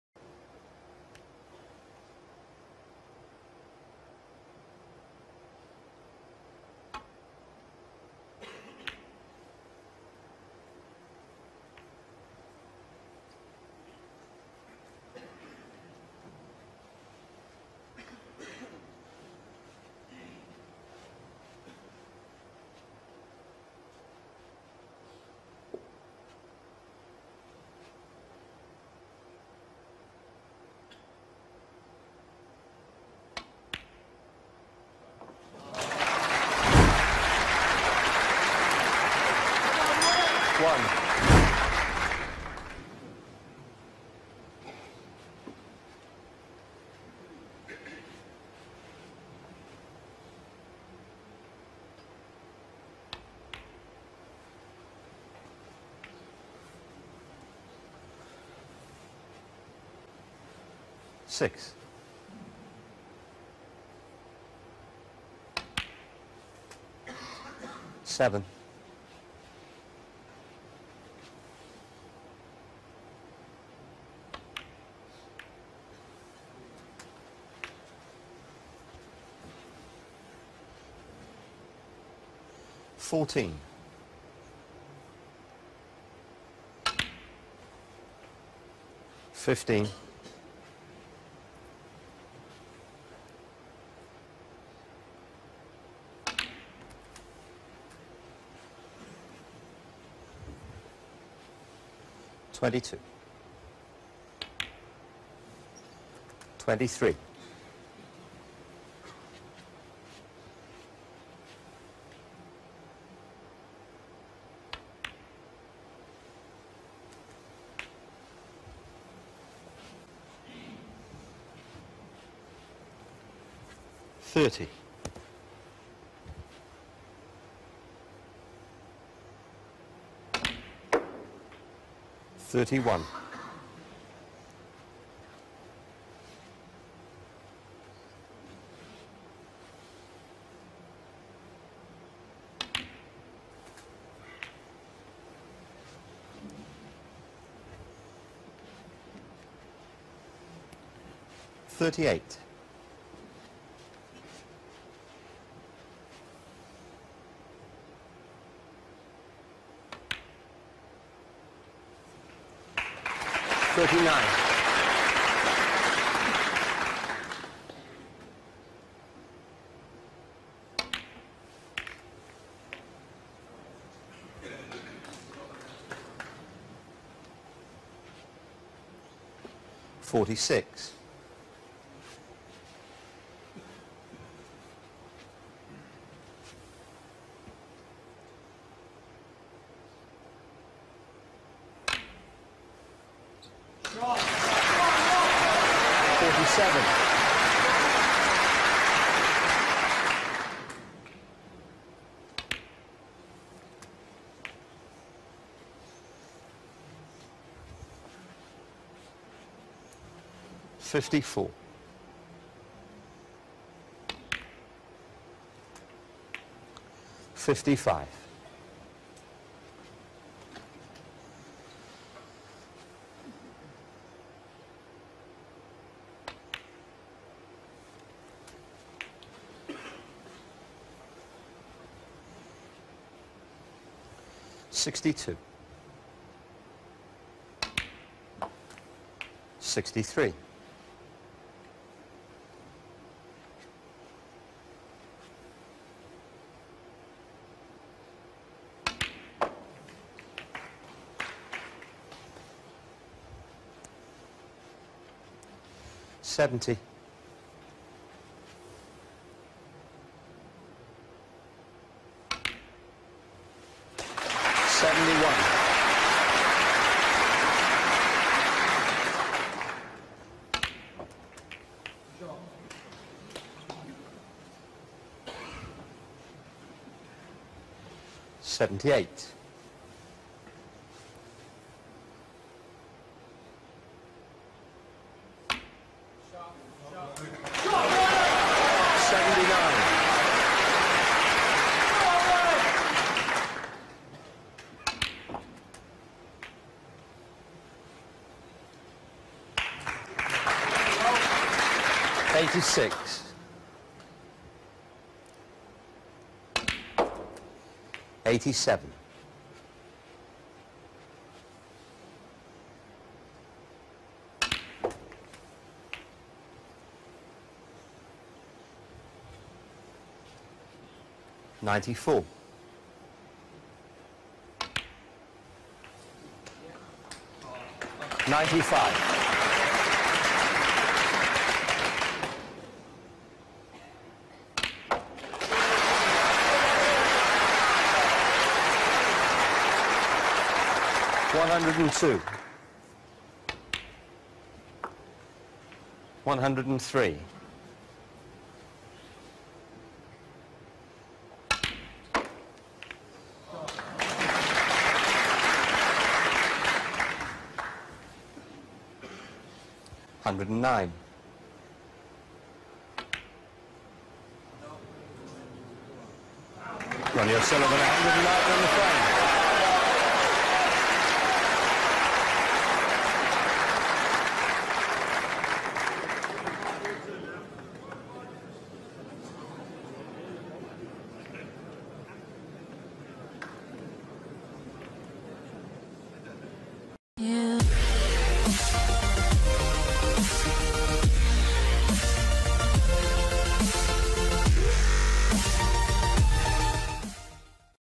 Ronnie O'Sullivan Simply The Best! Review in 2021!!!
Super game by Ronnie O'Sullivan in Champion of Champions in 2016 in the semi-final, let's review in now in 2021!
Do you like snooker matches? Or maybe you like to relax? :) By the way, the subscription to ADEN TUBE is Free!